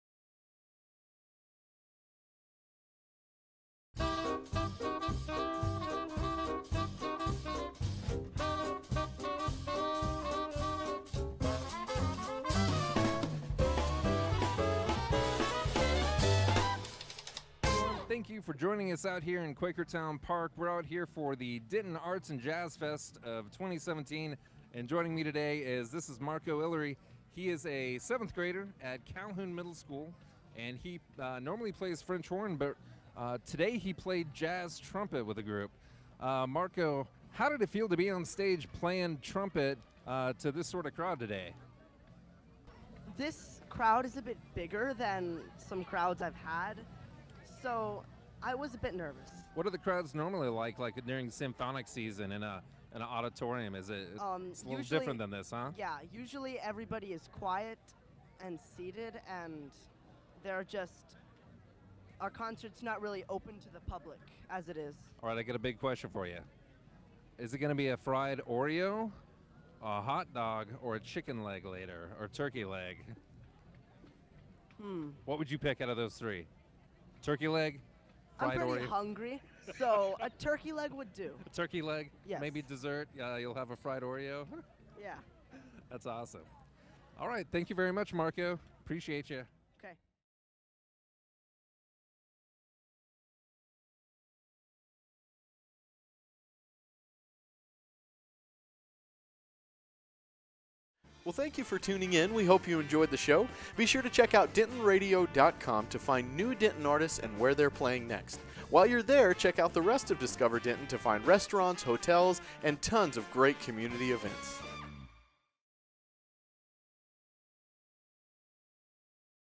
Denton Arts & Jazz Festival is underway!
tunein with Glen Farris of We Denton Do It to answer the burning question: hot dog, fried Oreo, or turkey leg?